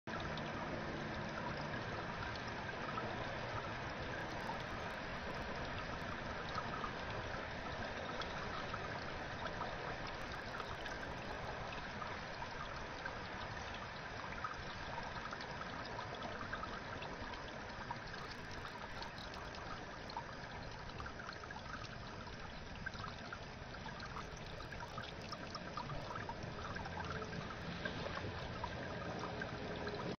Mr. Groundhog is testing my patience again
I love Nasturtium and this year, it grows like crazy all over the yard (but also in a fenced-off section in my veggie garden). I often gather some for salads or just to eat plain. It has a peppery flavor. I am surprised to see that the groundhog likes it too, but it also looks like it has to take a big sip right after. Once again, I know they are destructive but they just crack me up.